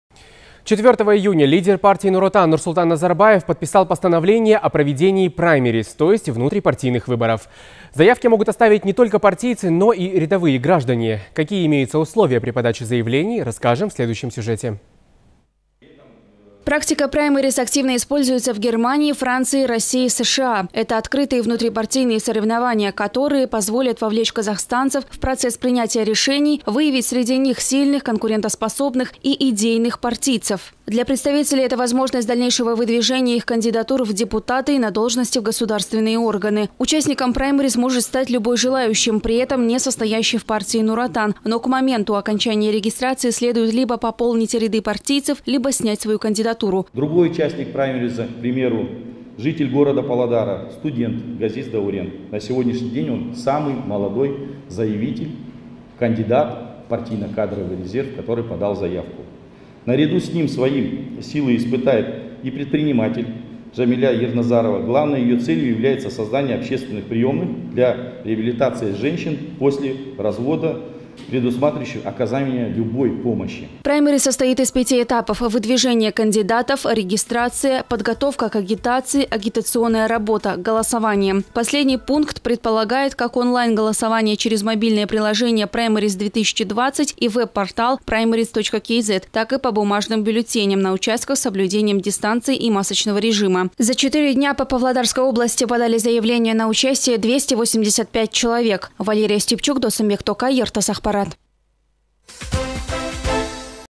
ПРАЙМЕРИЗ-2020
4 июня Лидер партии «Nur Otan» Нур-Султан Назарбаев подписал Постановление о проведении праймериз, то есть внутрипартийных выборов. Заявки могут оставить не только партийцы, но и рядовые граждане.